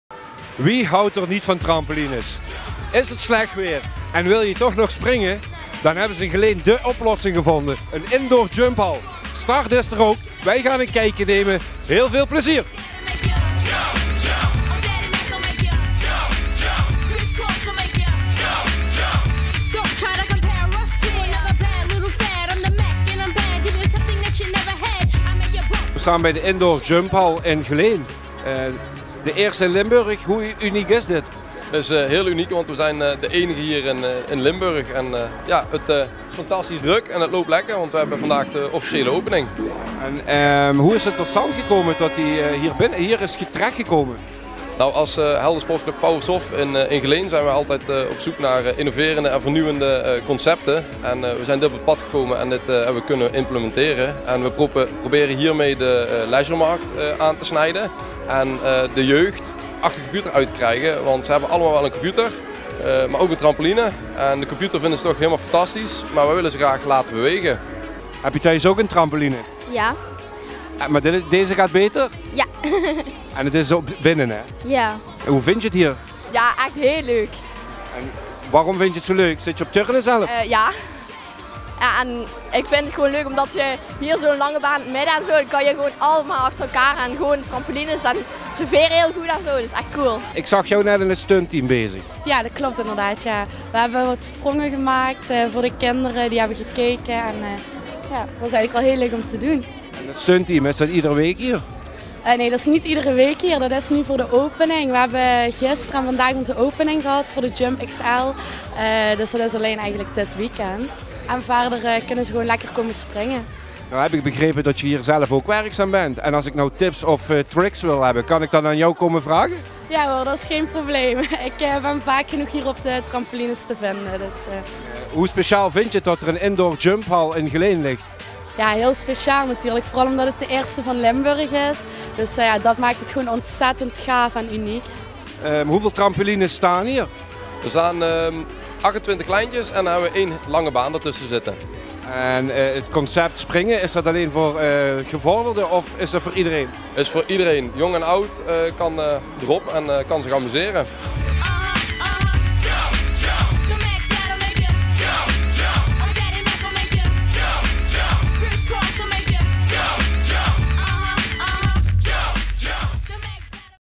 Opening Jump XL Geleen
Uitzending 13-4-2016

De indoor Jumphal aan de Vouershof Geleen werd op zondag 10 april 2016 geopend door burgemeester Sjraar Cox en wethouder Sportzaken Ruud Guyt.

Redactie & verslag: Tim Klein
Camera: Martin Heffels & Tim Klein
Montage: Martin Heffels

Eindredactie: Tim Klein

Wij zijn te zien op kanaal 41 van Ziggo, KPN kanaal 337/2140 en op de kabel zender 22+